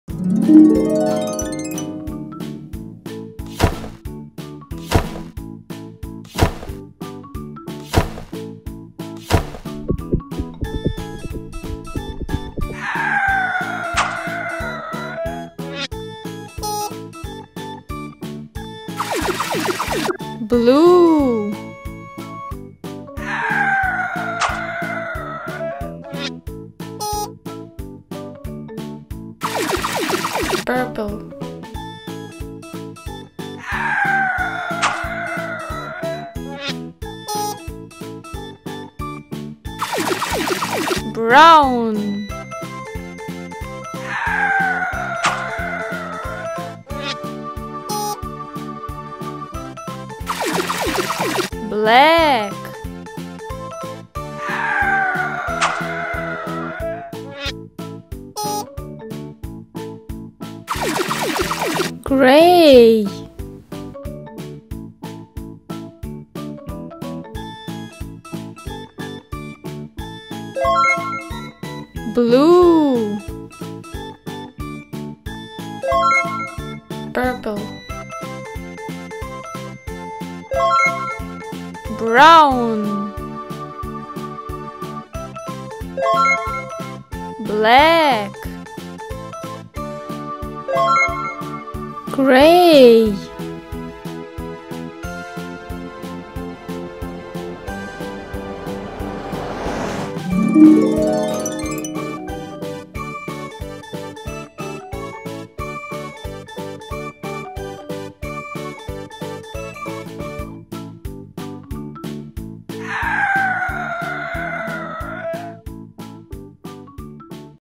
Learn Colors For Children With Chocolate Ice Cream | Educational Cartoons For Babies Kids Todllers
Make your kids enjoy the Chocolate Ice Cream. This video targets children, stimulating their imagination with the help of colorful objects. Each episode will help the child develop his or her creativity and logical reasoning.